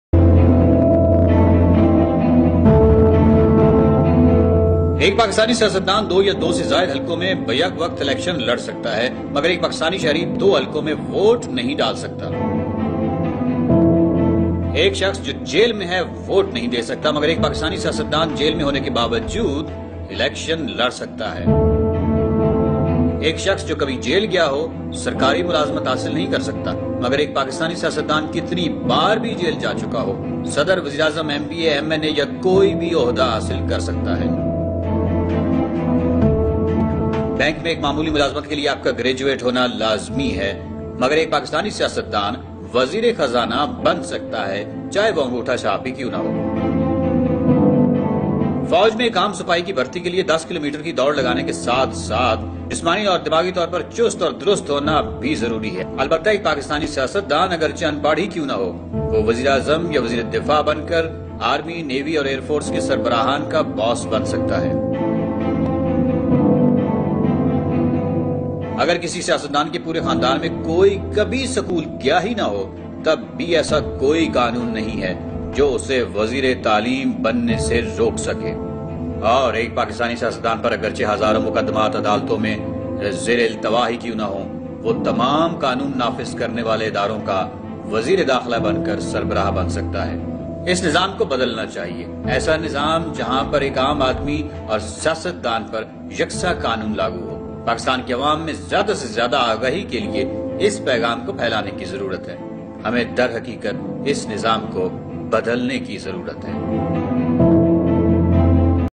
Pakistan's law |Blind law of pakistan
The Pakistani state has failed miserably in establishing justice.

Blind Law Of Pakistan.

امپورٹڈ_حکومت_نامنظور#